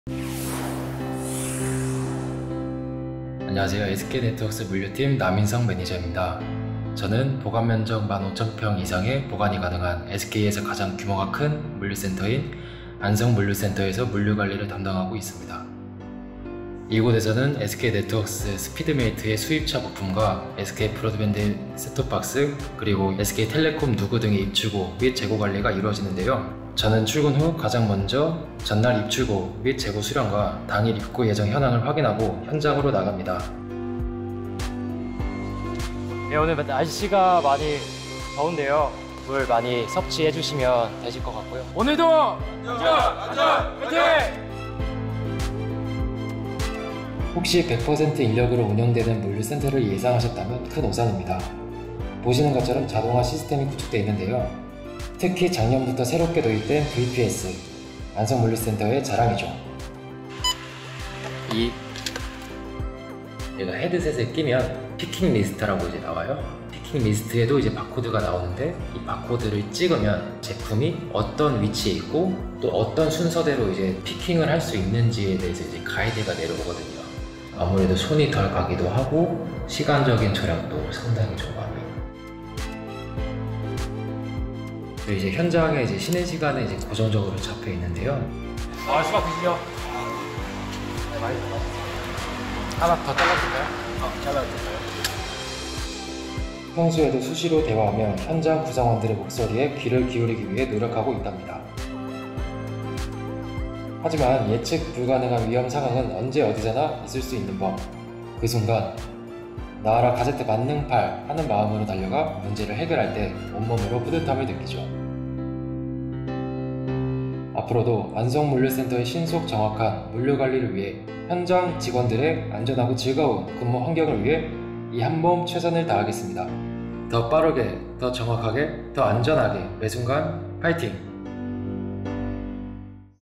[1인칭 SK人 시점] 자동화 시스템 VPS로 물류관리 걱정끝!
보관면적 1만 5천평 이상의 물류센터라도
VPS(Voice Picking System) 자동화 시스템으로
더욱 신속하고~ 간편하게 물류 관리 완료!

현장 구성원들과 즐거운 근무환경을
만들어나가는 SK네트웍스 물류팀을 만나보세요.